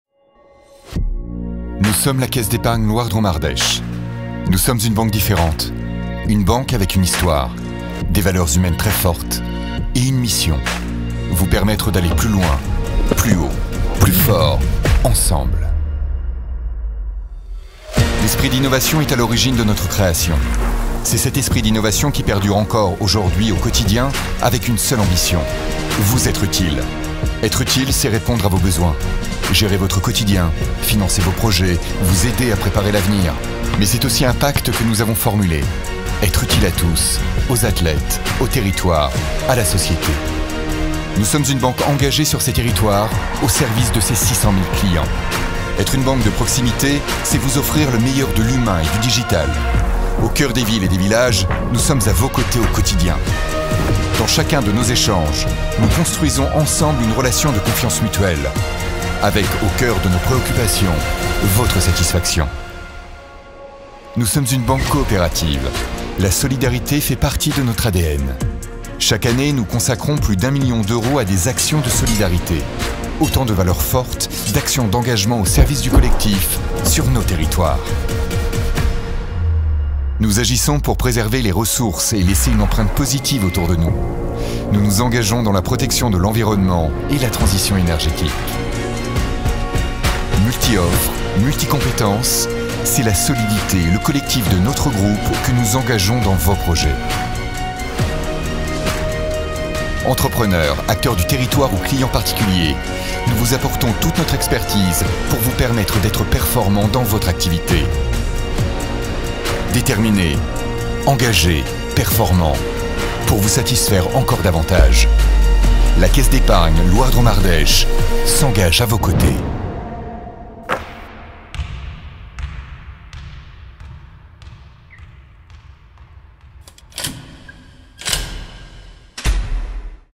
Film Institutionnel | Caisse d'Epargne LDA
La Caisse d’Epargne Loire Drôme Ardèche vous présente son nouveau film institutionnel.

Sport et banque, un parallèle fort entre deux univers, et autant de valeurs partagées qui définissent notre entreprise : utilité, innovation, engagement, confiance, performance… La Caisse d’Epargne LDA s’engage à vos côtés, avec une seule préoccupation : Vous Être Utile.